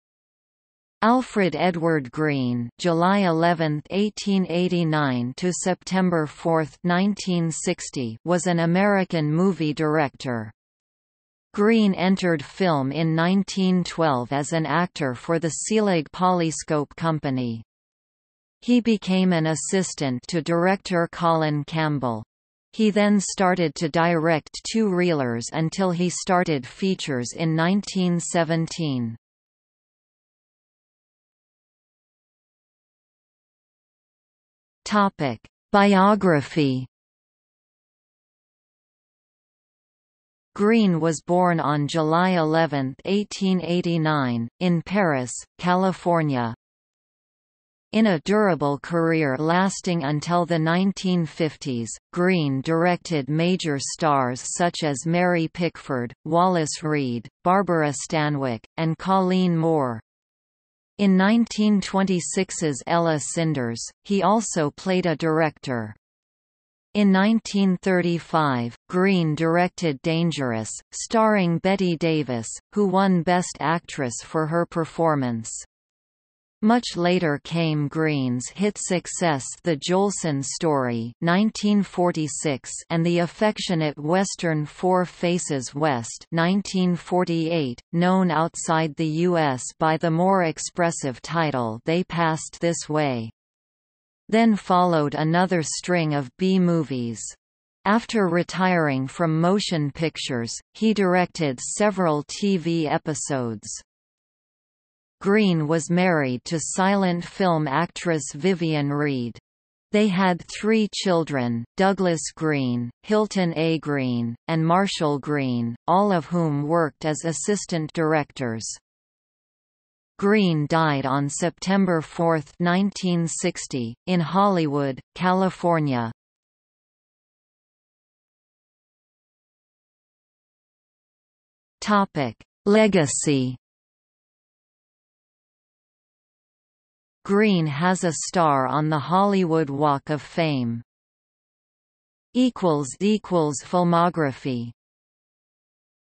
Alfred E. Green | Wikipedia audio article
00:00:31 1 Biography
00:02:08 2 Legacy

- Socrates

SUMMARY
Alfred Edward Green (July 11, 1889 – September 4, 1960) was an American movie director. Green entered film in 1912 as an actor for the Selig Polyscope Company. He became an assistant to director Colin Campbell. He then started to direct two-reelers until he started features in 1917.